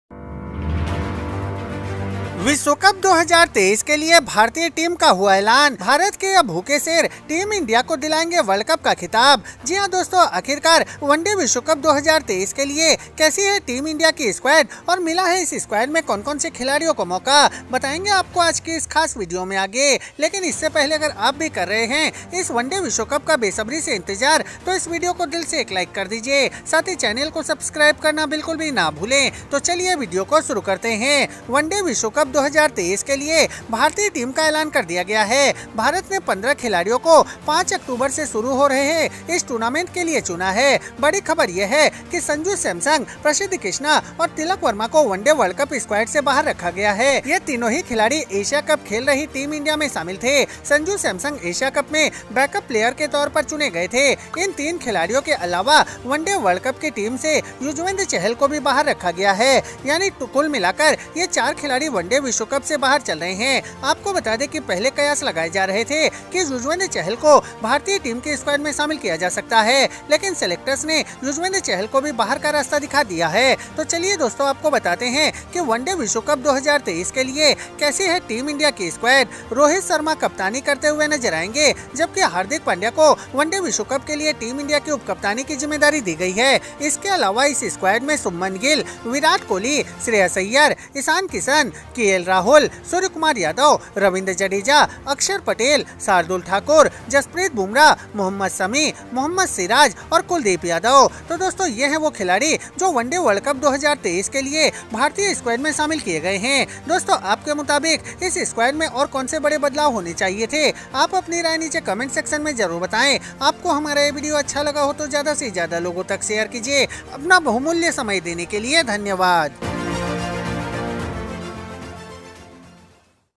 Breaking News | World cup 2023 Team India Squad
Breaking News -विश्व कप 2023 के लिए भारतीय टीम का हुआ ऐलान | World cup 2023 Team India Squad